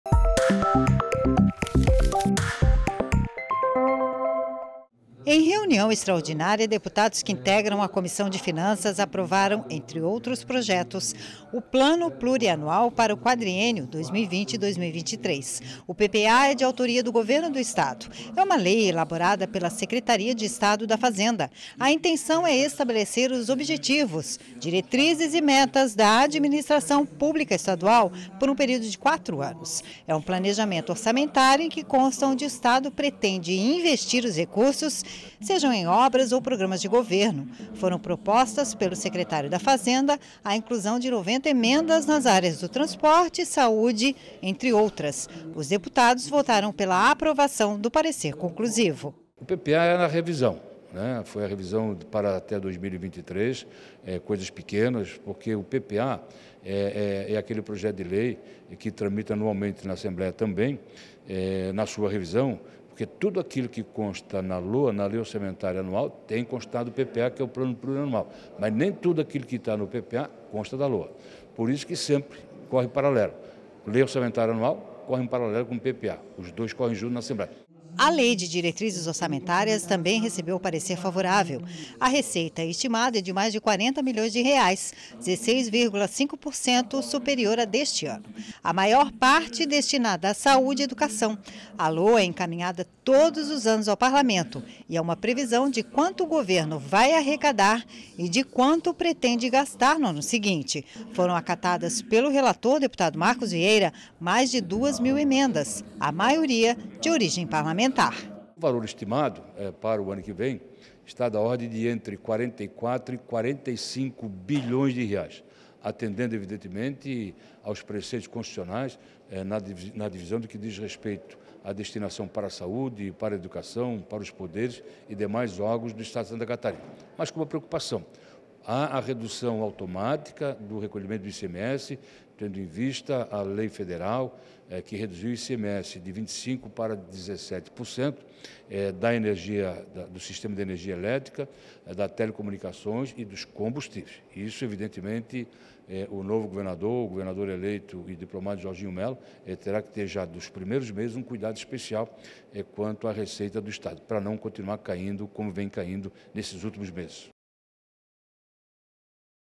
É Notícia - Plano Plurianual e LDO são aprovados na Comissão de Finanças - 20/12/22
A Comissão de Finanças aprovou nesta terça-feira o Plano Plurianual para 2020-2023, além da Lei de Diretrizes Orçamentárias. A receita estimada para o ano que vém é de mais de 40 milhões de reais.